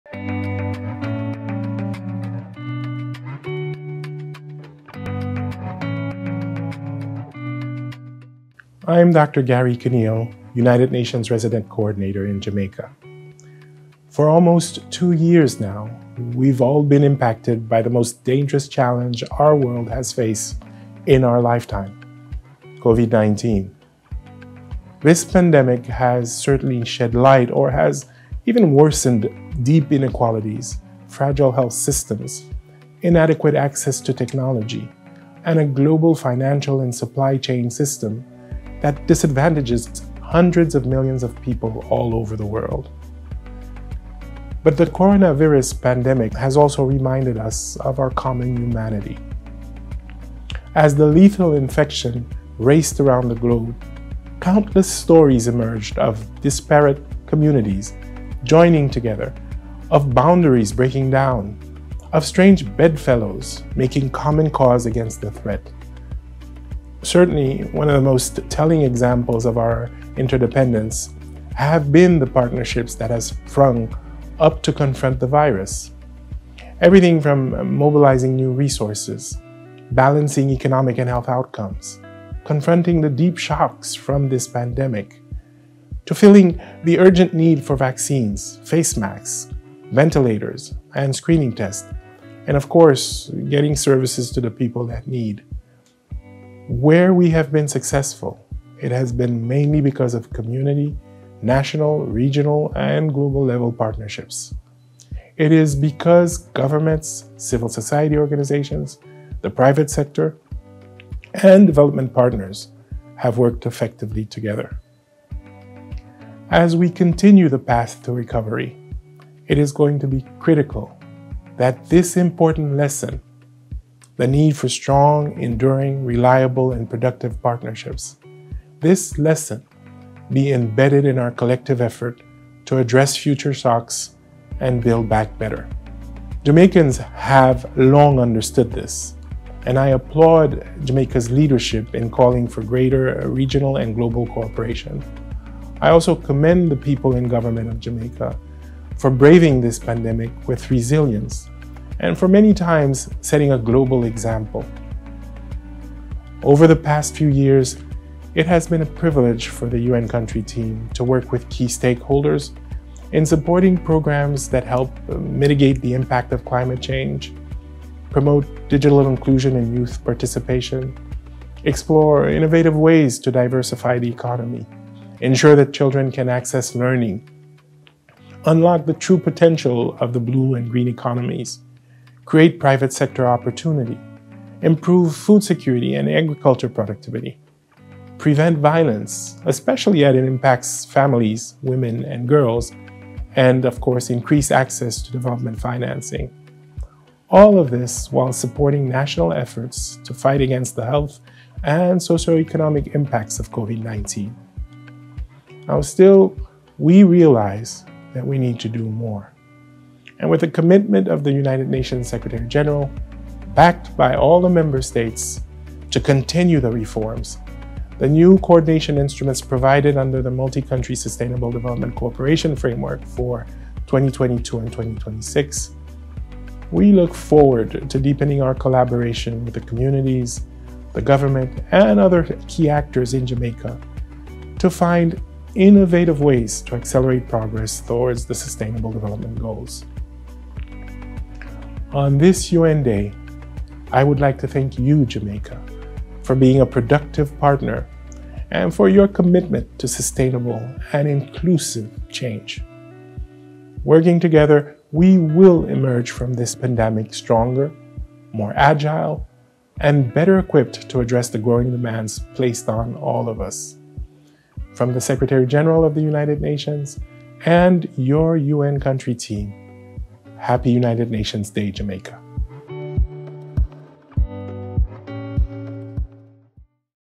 UN Day Message Dr. Garry Conille, Resident Coordinator
The official message of the UN Resident Coordinator in Jamaica on the occasion of Oct. 24, United Nations Day 2021.